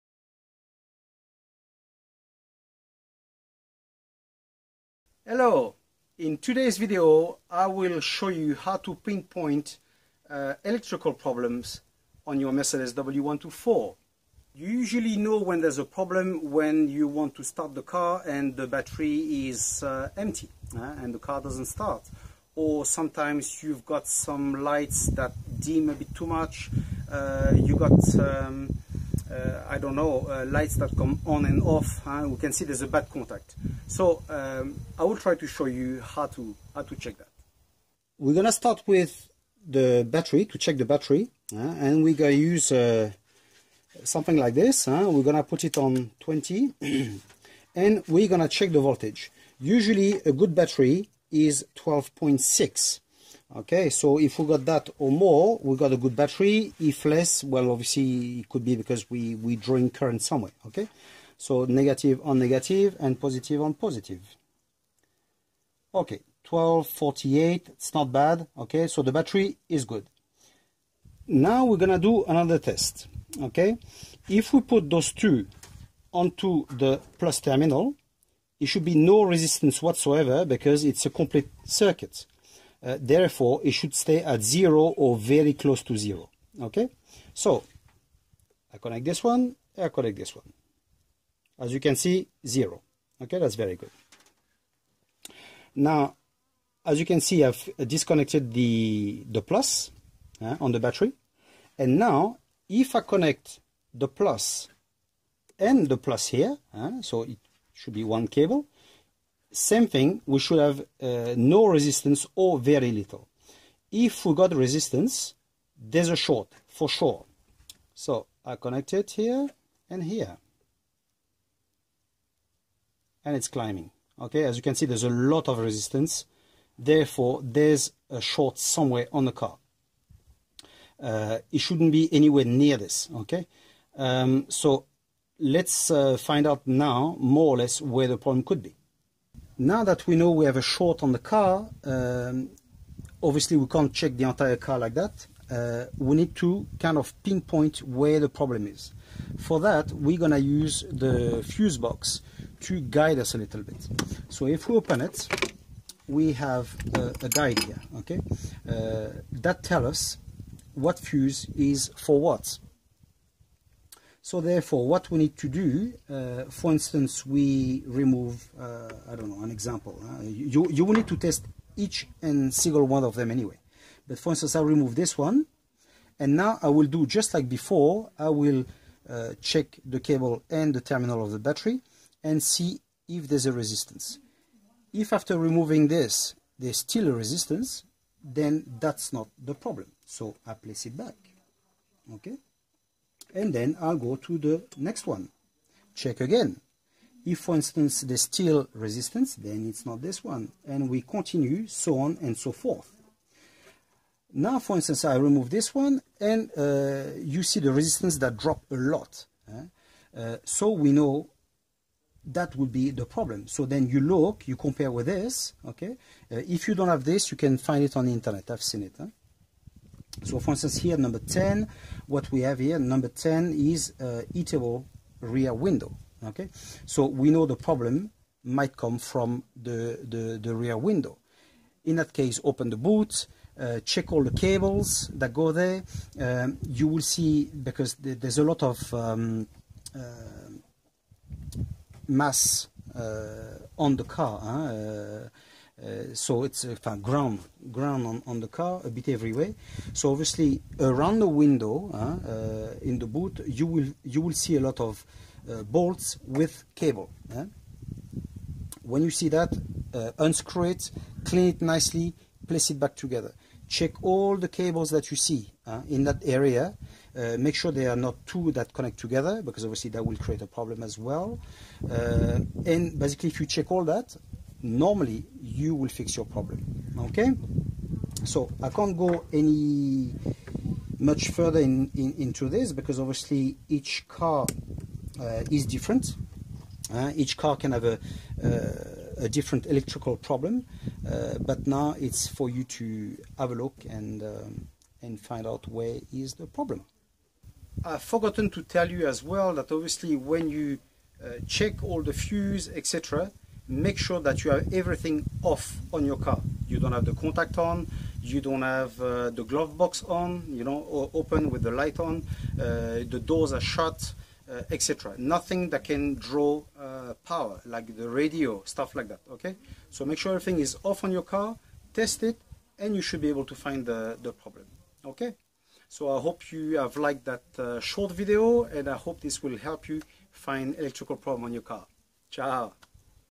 Mercedes Benz W124 - How to diagnose / find electrical faults tutorial DIY
Maintenance and keeping your Mercedes Benz in good shape.

I am Hervé Collette and I will show you in this video how to diagnose electrical faults on a Mercedes Class E, W124.

If you would like to help me financially with this channel, you can do so using PayPal.

Hope this will teach you something with this DIY tutorial so you won´t have to take your car to the workshop, save some money and keep your Mercedes on the road.